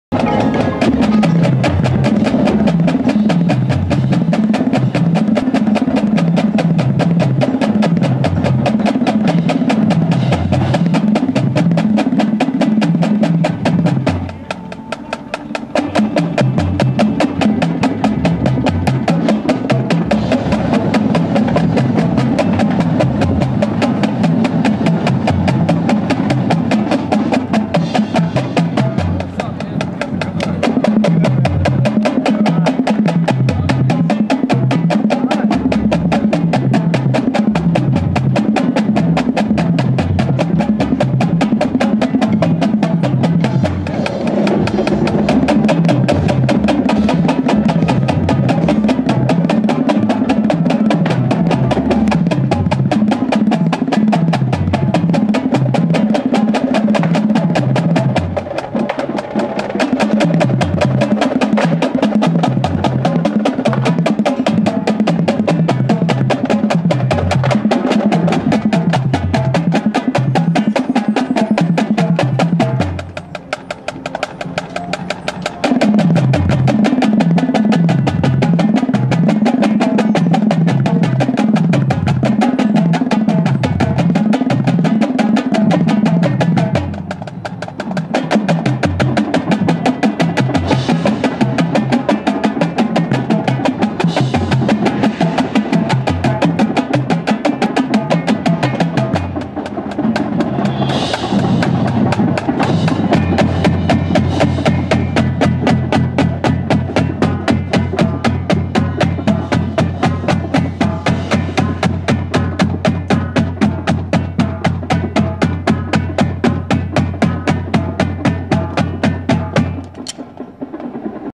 2009 Blue Knights 8's WGI finals
2009 Blue Knights World Percussion warming up in the lot at WGI prelims in Dayton, Ohio.